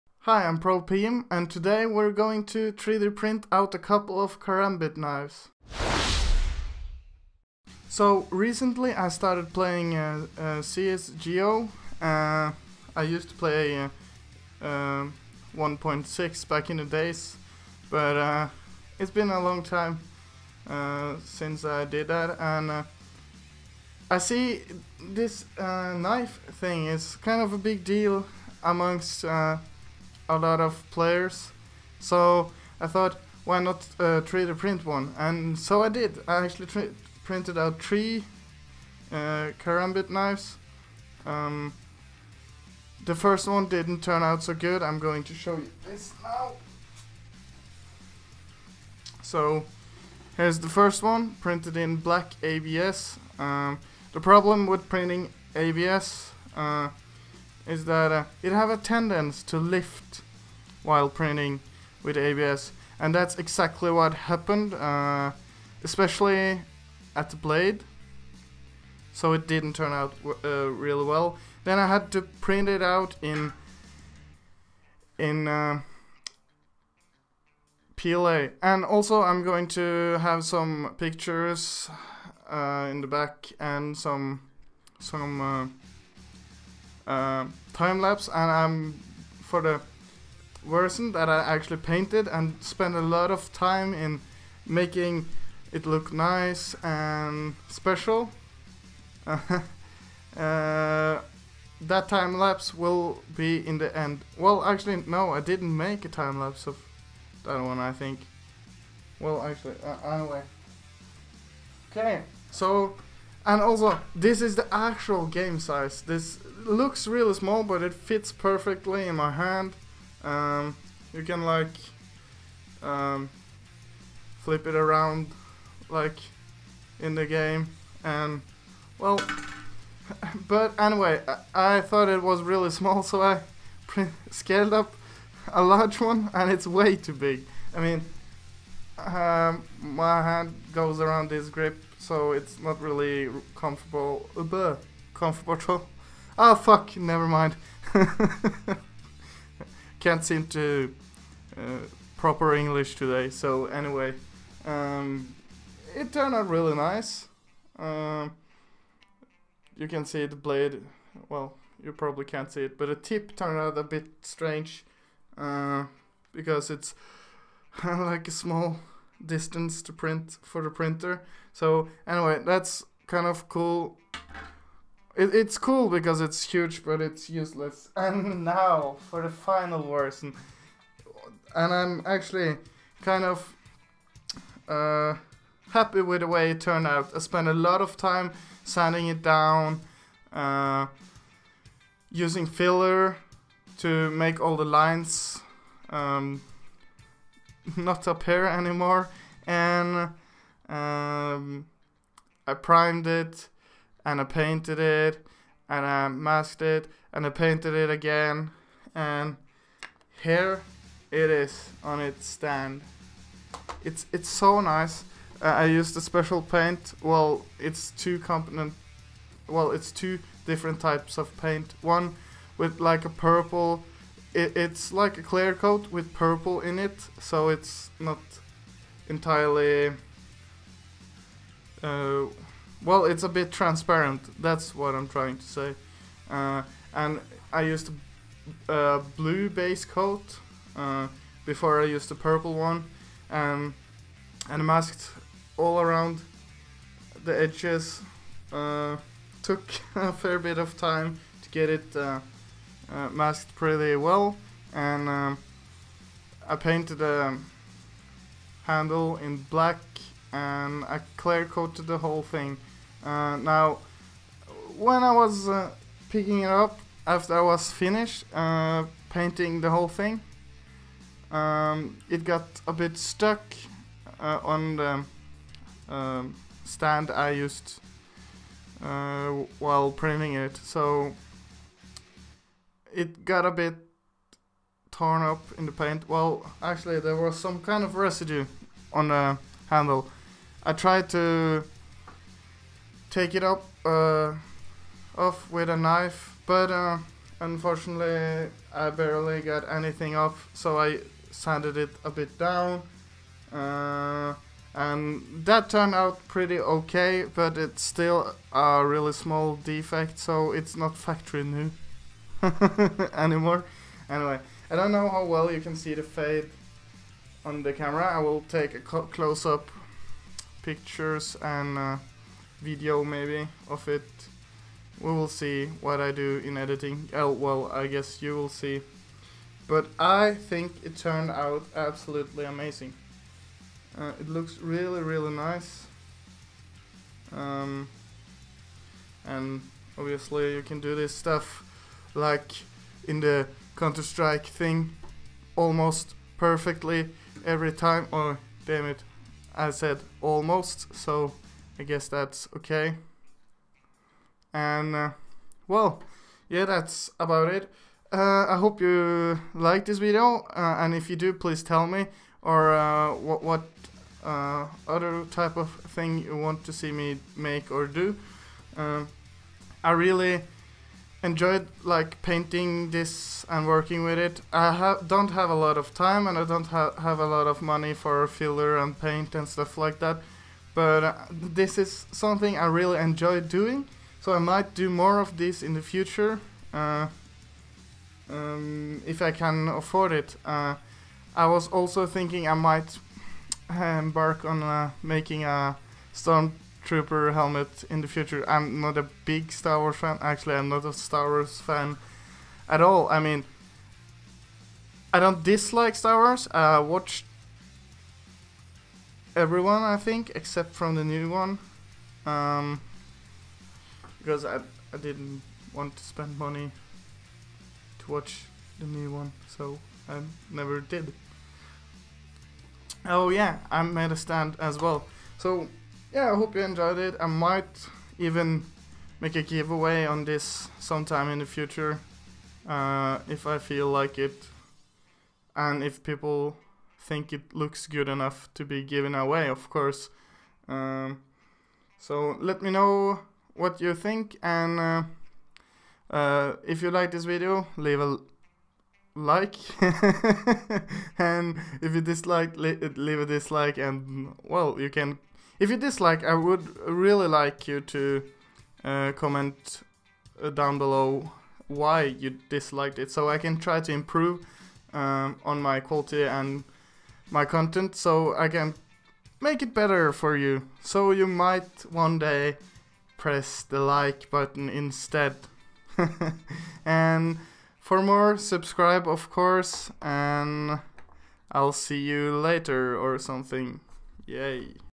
CS:GO Karambit Knife - 3D printing and painting
So I recently started playing CS:GO and since I do 3-D printing I should mix em up right? And I did, and printed out a Karambit knife that I also did a lot off work on to get a nice finish and a good looking show piece

and as always, thanks a lot to teknoAXE for his awesome music, check it out